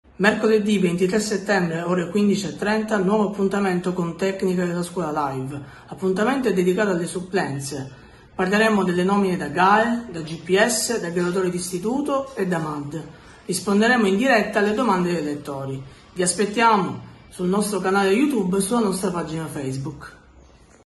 Speciale supplenze, alle 15:30 segui la diretta con tutti i chiarimenti
Tutto sulle convocazioni da Gps, Gae, Graduatorie di istituto e Mad, i nostri esperti saranno a tua disposizione per rispondere alle tue domande.
Consulta il sito per essere sempre aggiornato sulle notizie del mondo della scuola.